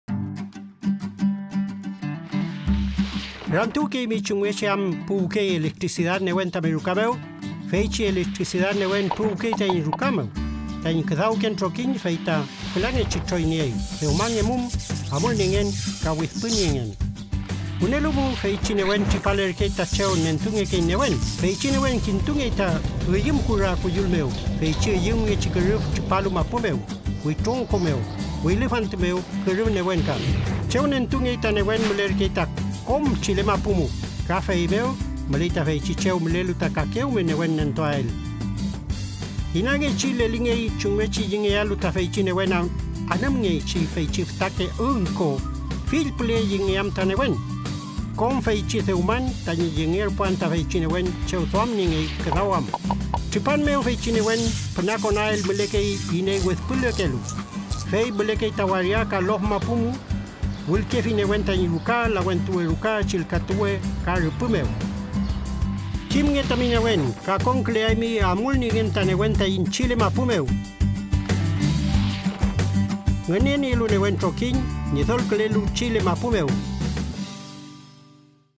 Conoce Tu Energía - ¿Cómo llega la electricidad a tu hogar? (Mapudungún)
La electricidad llega a nuestros hogares, oficinas o industrias en un proceso que se divide en tres partes: generación, transmisión y distribución.

Aprende y accede a más iniciativas, recursos educativos y otras oportunidades en el mundo de la energía